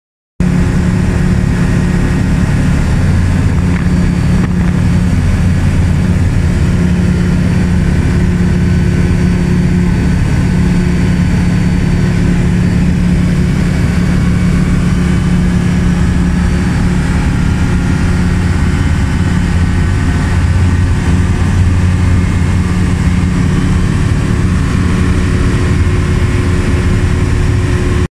Über Miltenberg mit tollem Ausblick auf den Spessart und Odenwald
Das kurze Video entstand in etwa 3000ft auf südlichöstlichem Kurs über Miltenberg mit Blick auf Miltenberg, Erlenbach, Else Feld, den Spessart und den Odenwald...

Rundflug, fliegen, aus der Luft, Flugzeug, luftaufnahme